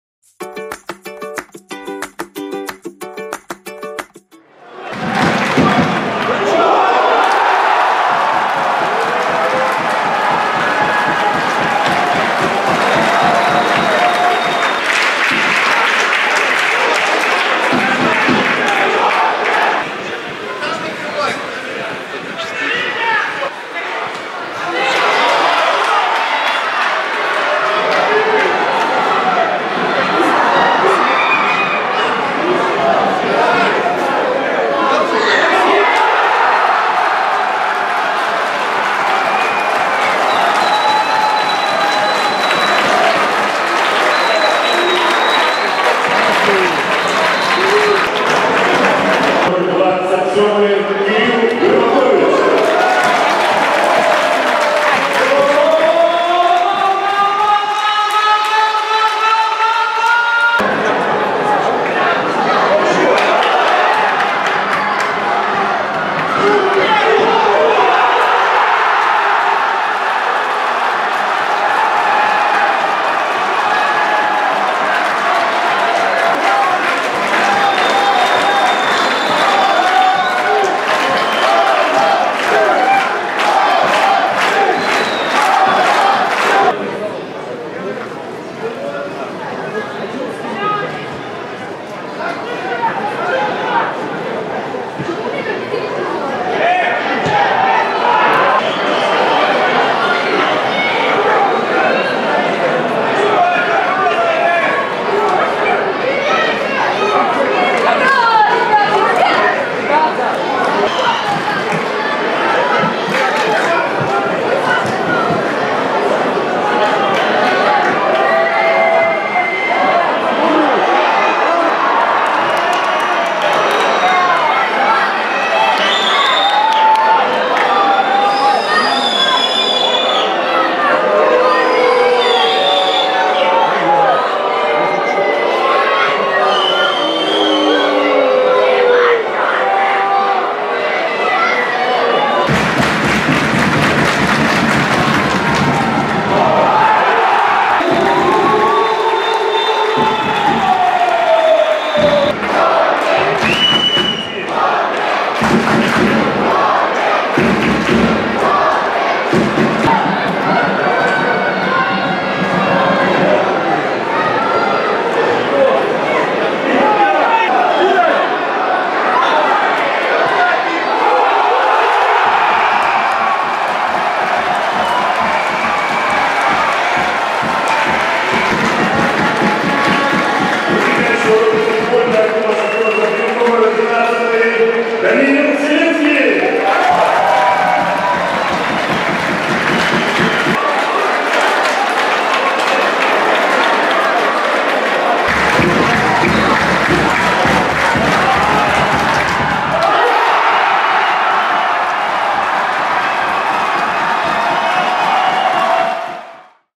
Голосование за лучший гол августа
Собрали для вас подборку всех забитых мячей нашей команды в августе. Давайте вместе определим лучший гол прошлого месяца.

Голосование проходит в нашей группе «Вконтакте» и Telegram-канале.